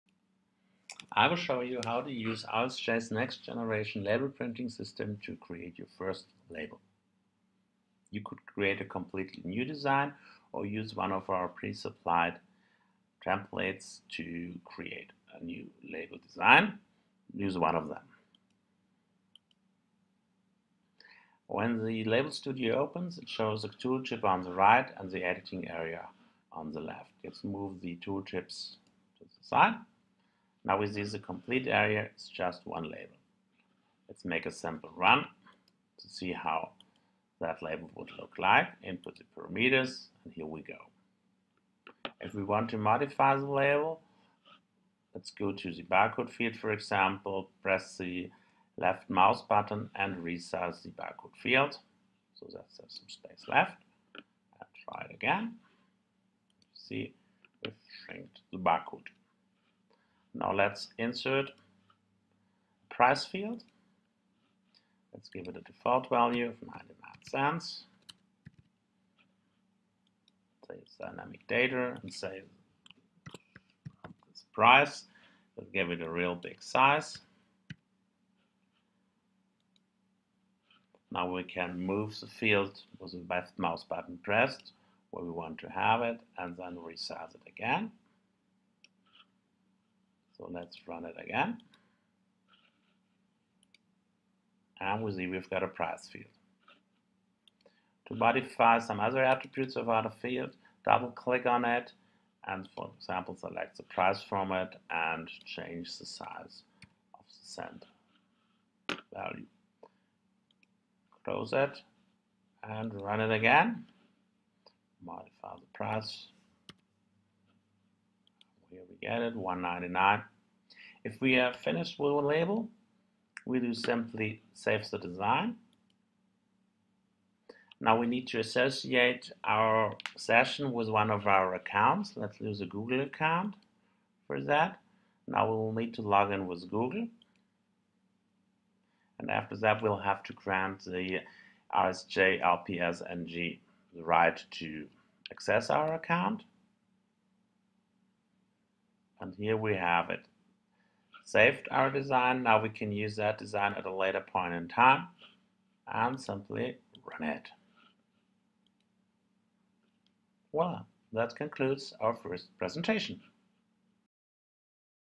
How to create your first labels with RSJ Next Generation Label Printing System
Screencast of basic functionality from http:// lpsng.rsj. de